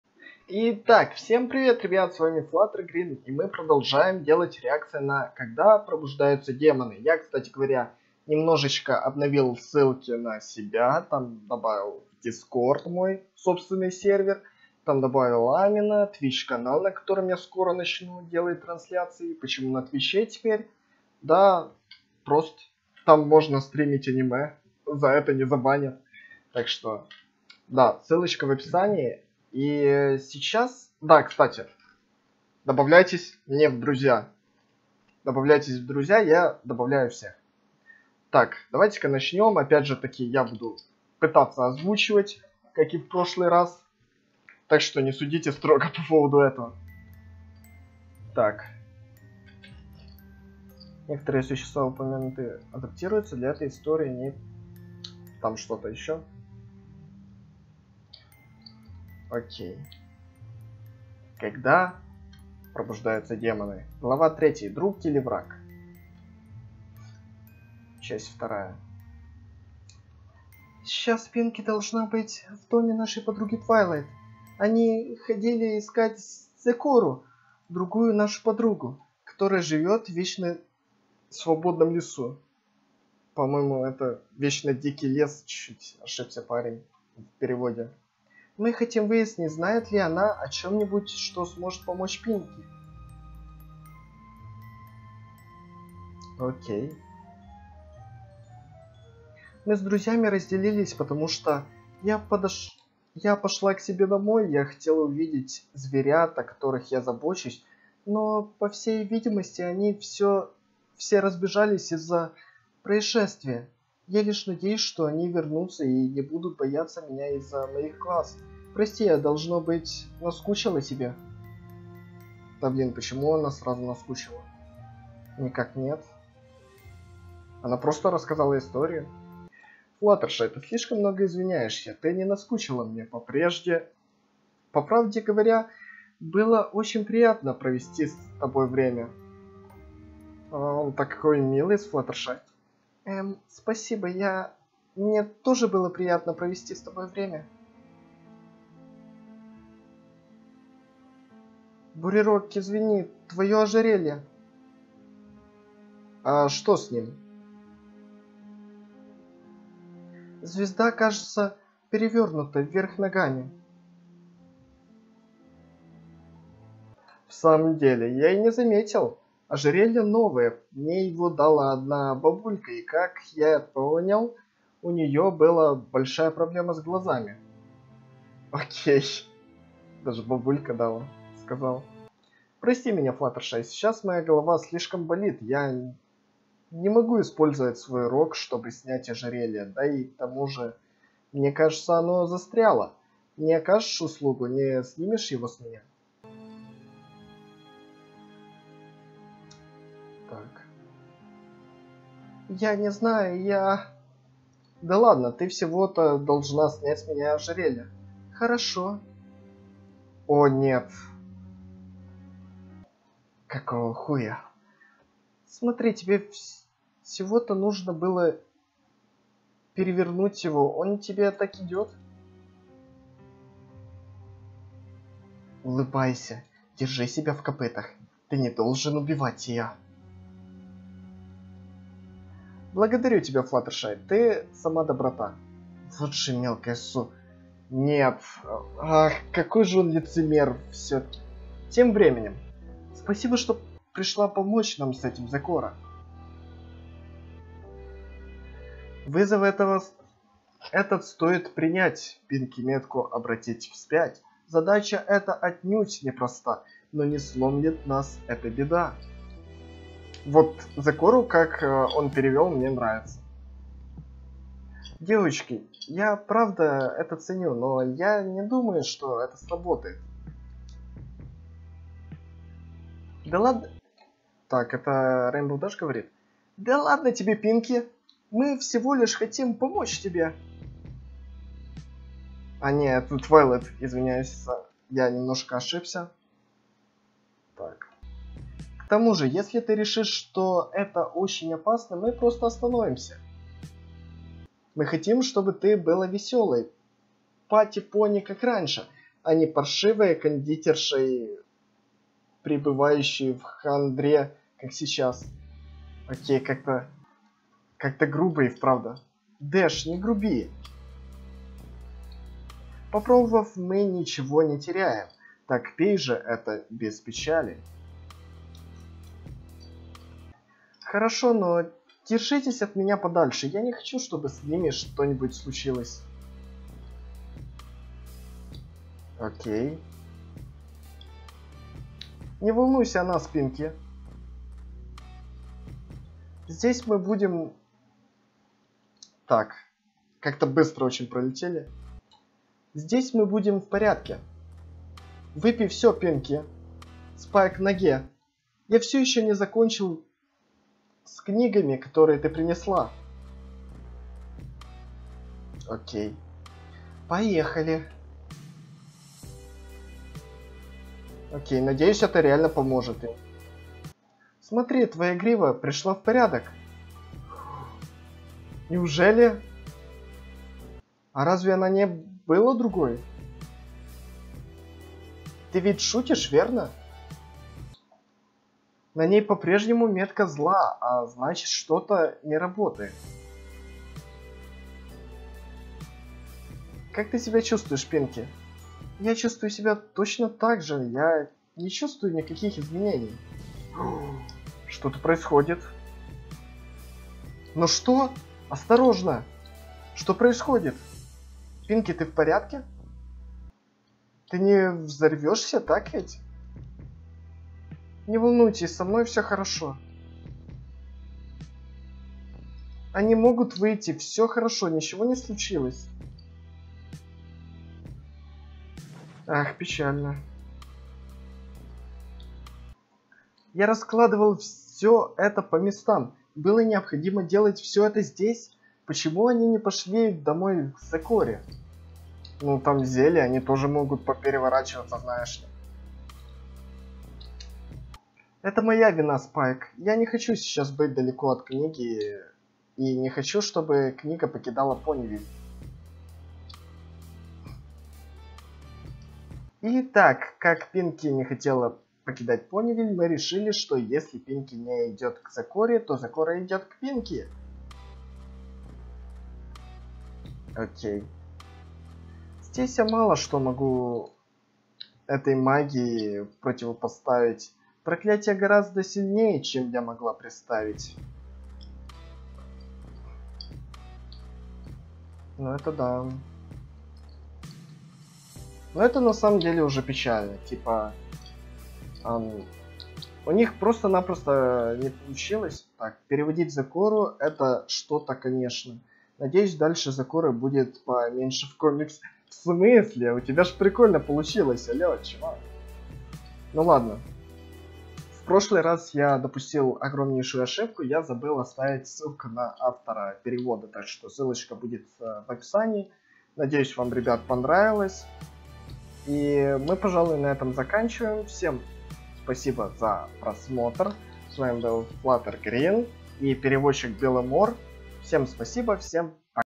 Когда демоны пробуждаются / When Demons Awake / Часть 5 - Реакция на комикс
Реакция Флаттер Грина на комикс When Demons Awake┃Когда демоны пробуждаются MLP┃ Крипипаста┃

-- Найти меня! --